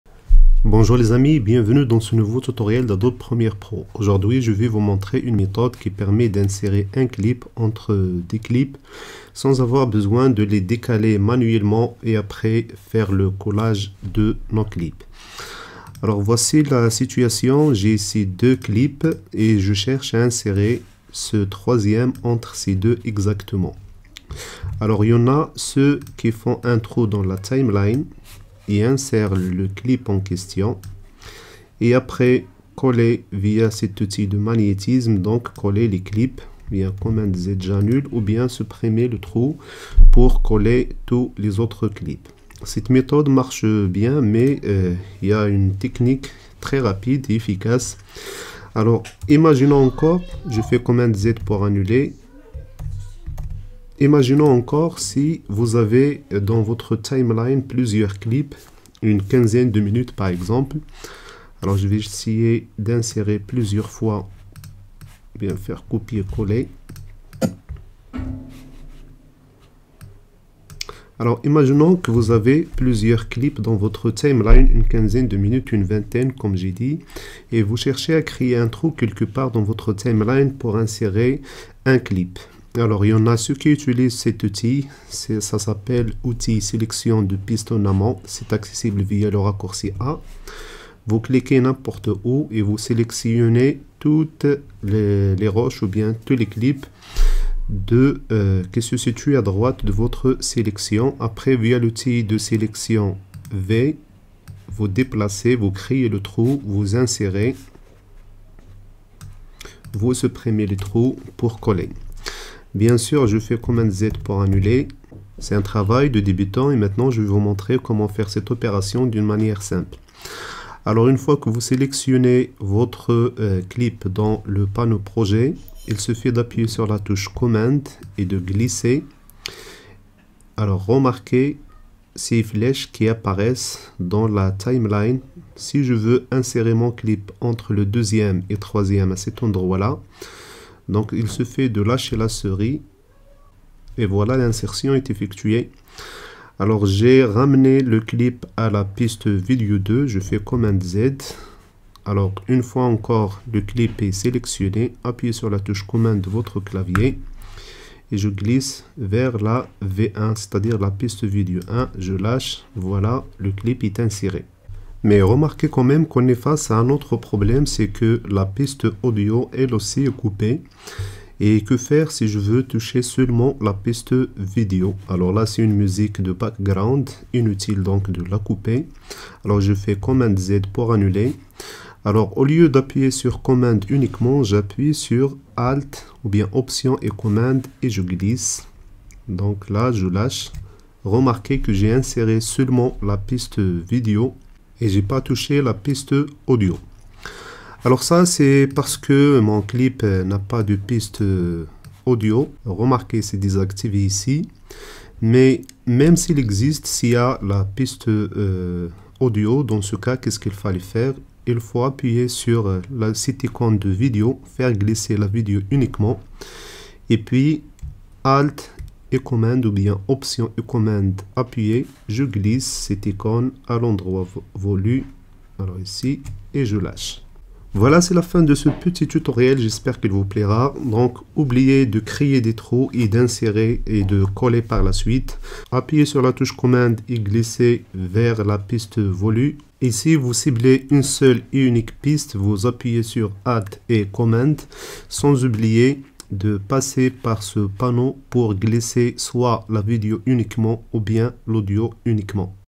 Insérer un clip entre 2 autres avec prolongement automatique
Pour insérer un clip entre deux autre et prolonger automatiquement dans la timeline, inutile de créer des trous, d'insérer et ensuite coller. il suffit d'appuyer sur la touche "Commande" de votre clavier au moment que vous glissez le clip dans la timeline. et pour cibler une piste unique il suffit d'appuyer en plus de "Commande" sur "Option".